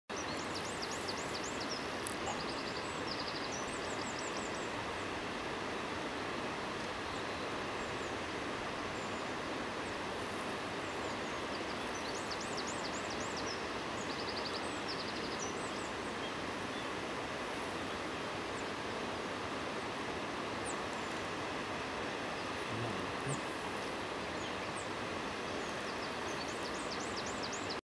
Cervi nel Parco nazionale d'Abruzzo
Passaggio di un branco di cervi. L'ultimo mi vede e si ferma anche se ero abbastanza lontano.